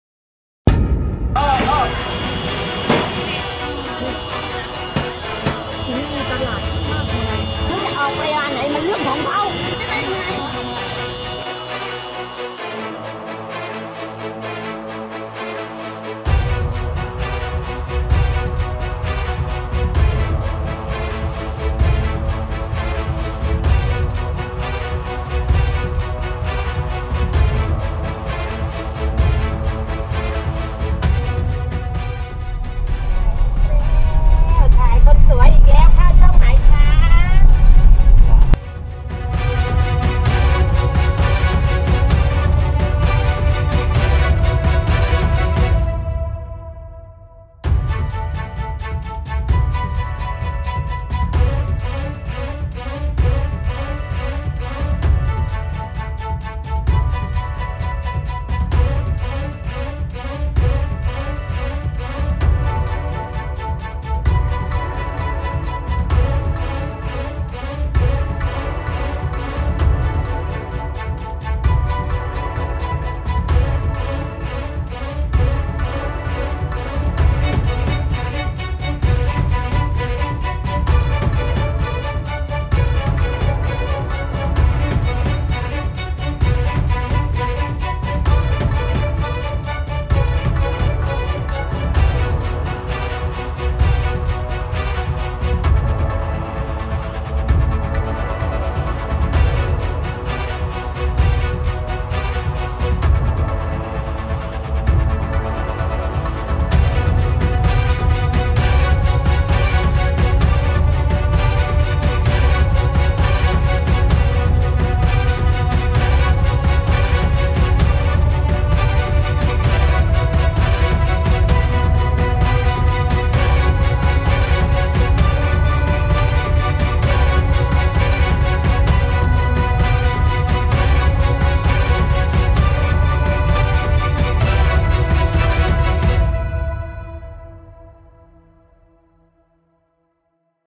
พาสำรวจตลาด "บ้านป้าทุบรถ" หลังผู้ค้าตั้งตลาดแห่งใหม่
ครบรอบ 1 เดือน หลังข่าวป้าทุบรถตกเป็นกระแสตั้งแต่วันที่คลิปเผยแพร่เมื่อวันที่ 19 ก.พ.61

สถานการณ์วันนี้จะเป็นอย่างไร? พาสำรวจตลาด "บ้านป้าทุบรถ" หลังผู้ค้าตั้งตลาดแห่งใหม่